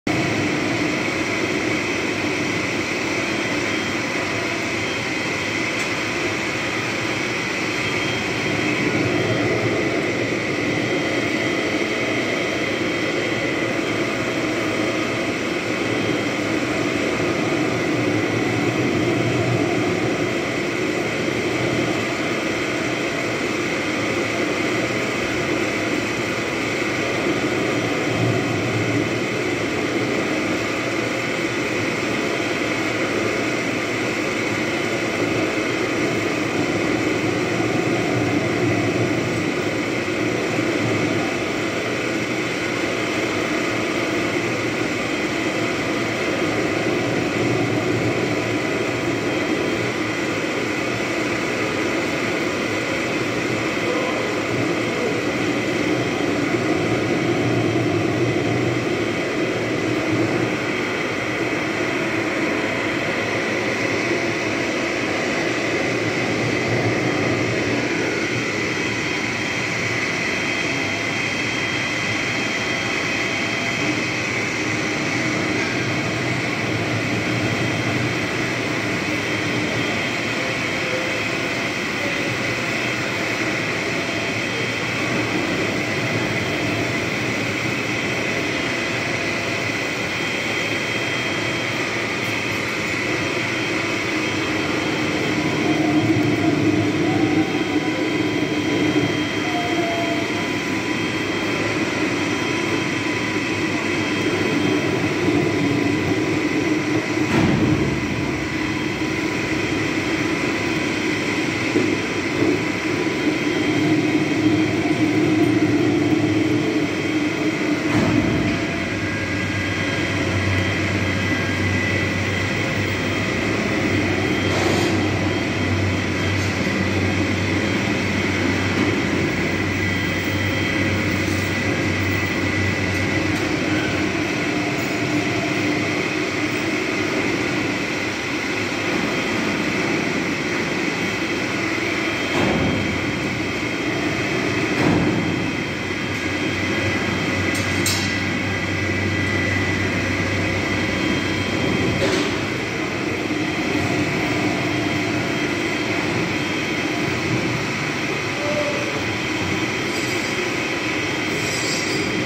Let's have a zoom on cnc lathe machine, lathe operation asmr
my job is something entertaining for me and I really enjoy  although is some kind of difficult. but I try to upload interesting videos of how I'm working.